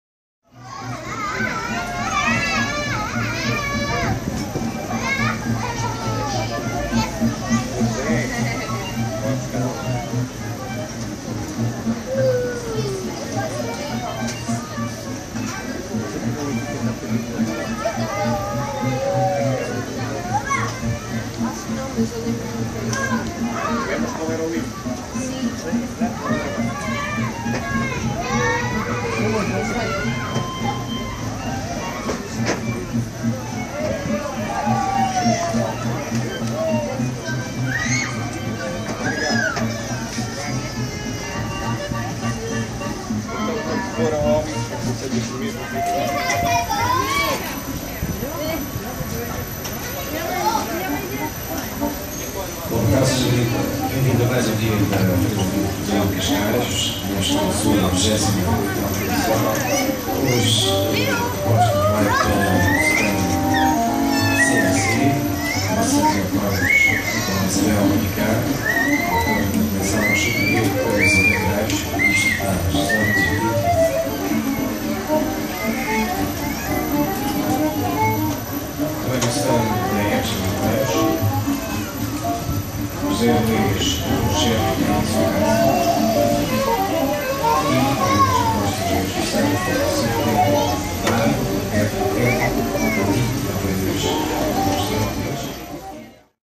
Galaxy S4 Video Sample (1080p) - Merry-go-round
Recorded handheld at 1080p, using the built-in microphone.